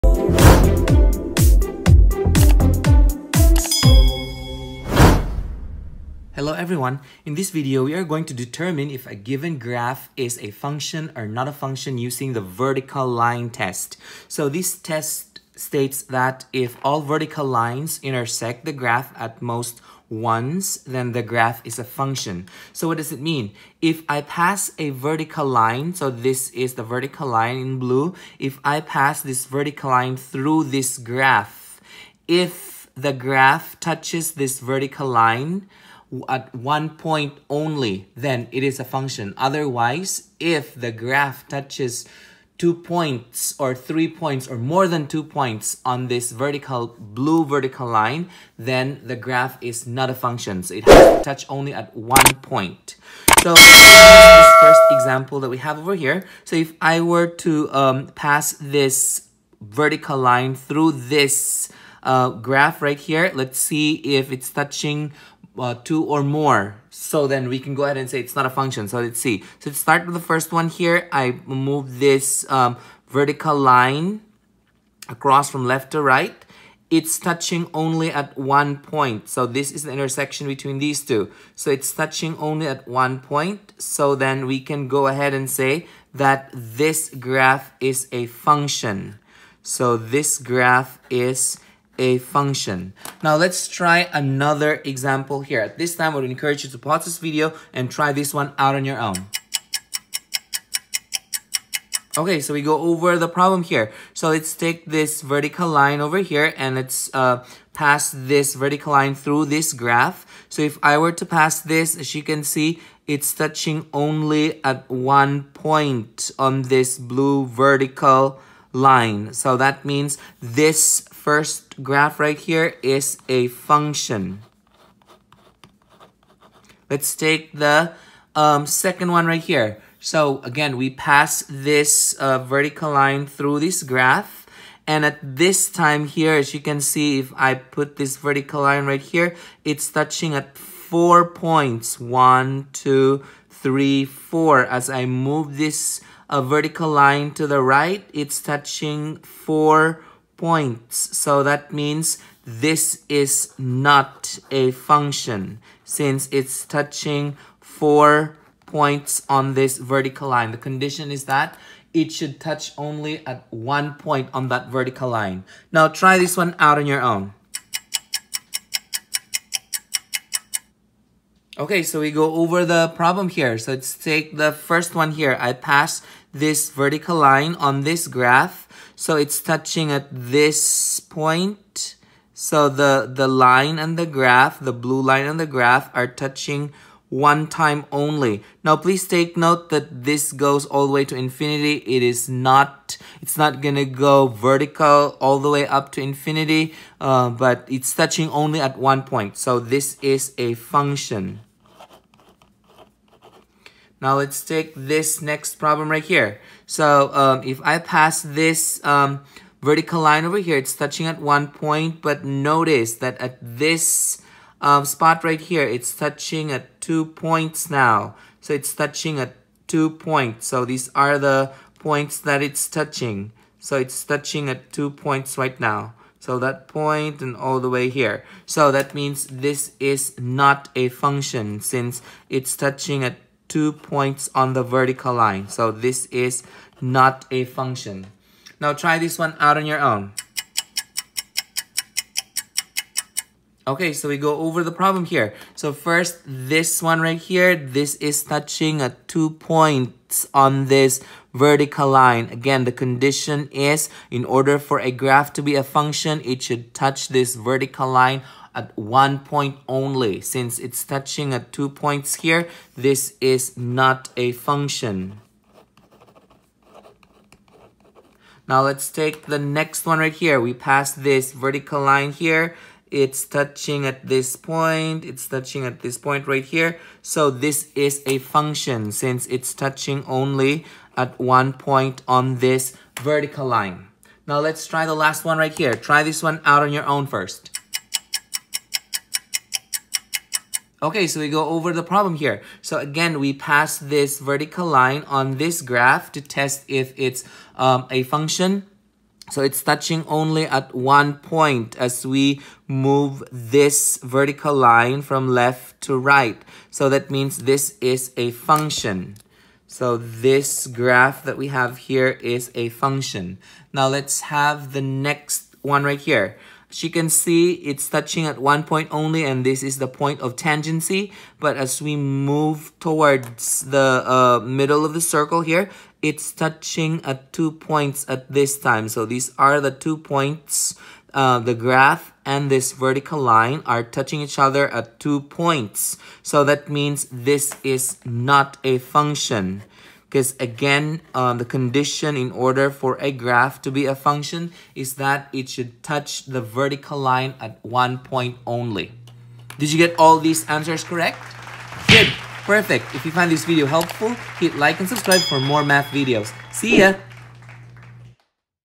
Is the Given Graph a Function or Not a Function???
This video will show how to use the vertical line test to determine whether the given graph is a function or not a function